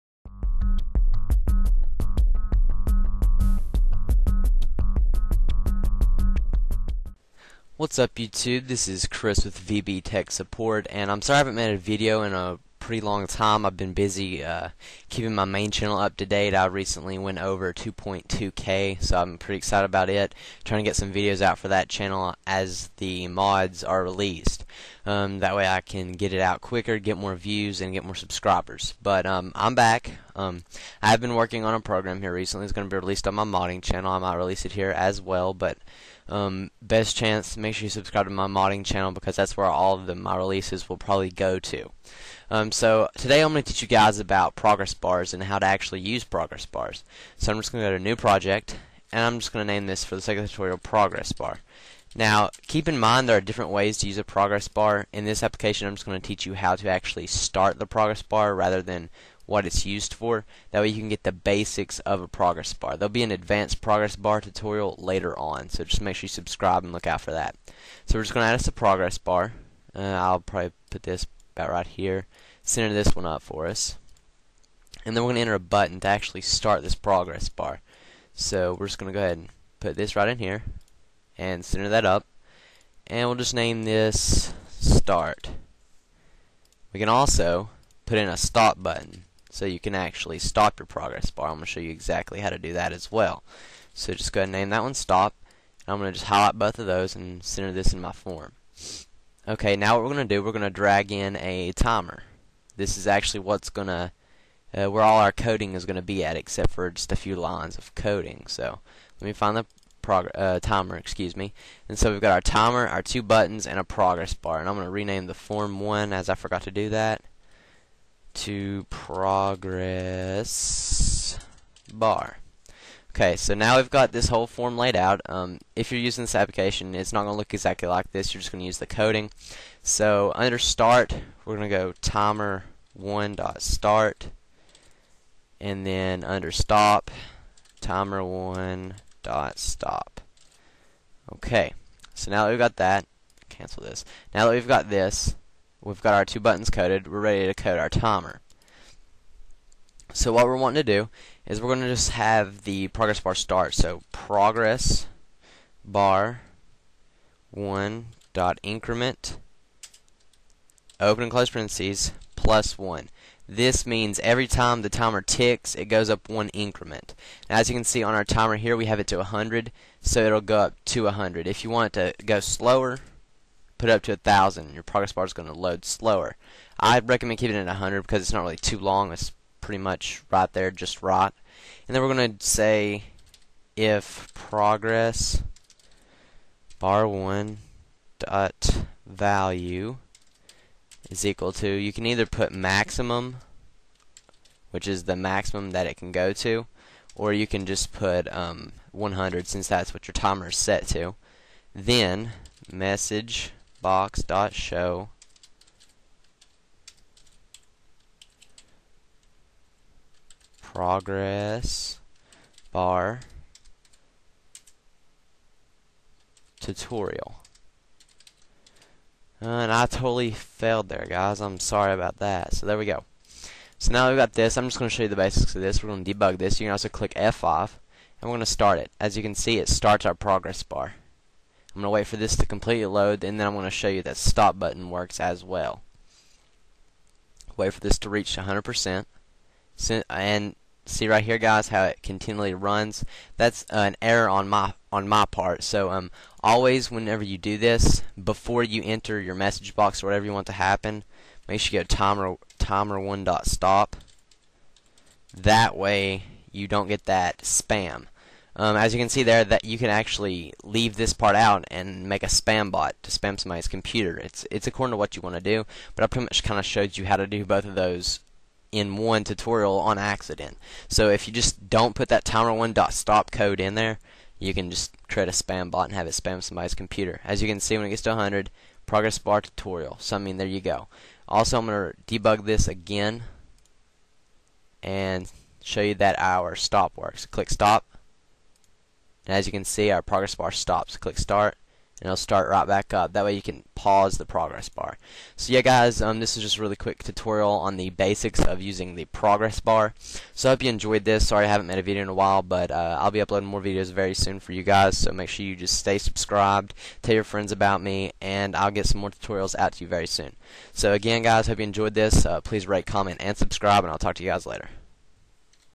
VB 2010 Tutorial #5 | Basic Progress Bar
In this video tutorial I will teach you how to make a progressbar function withing your application. I do not get into much detail about all the progressbar can do inside of an application as I am saving the more advanced things for a later tutorial on Advanced ProgressBars. I hope you enjoy this tutorial and you will find someway to implement this into your very own applications!